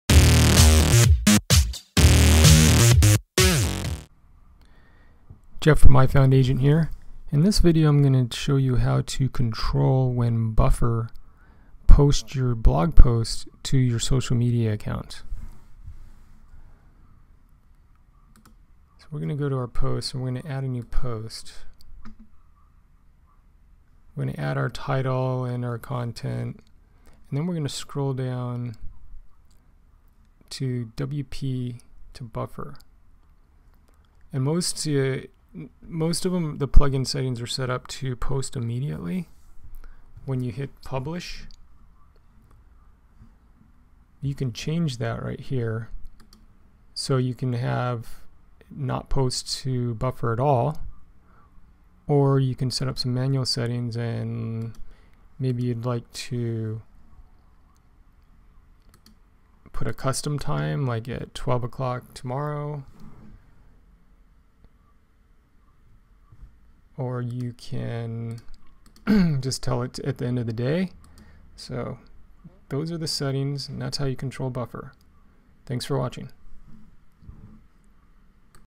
WP to Buffer Settings
This video shows how to control when and if you want to post a blog post to social media using Buffer